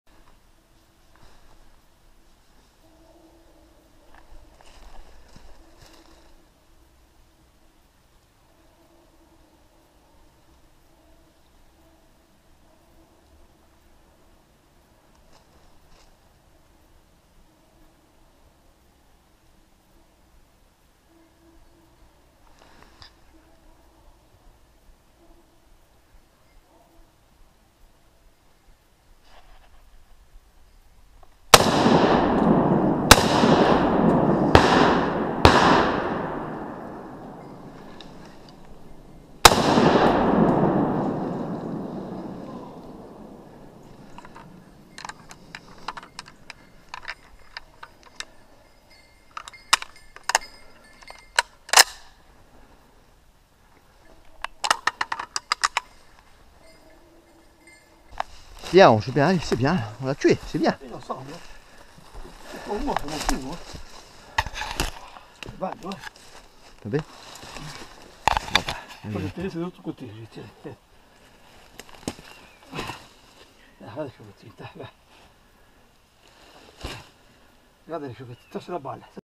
chasse au sanglier en corse 2017
un petit loupé, le sanglier revient de derrière je le loupe et le poste en dessous le tir et le tue bien joue ange sont troisième du week- end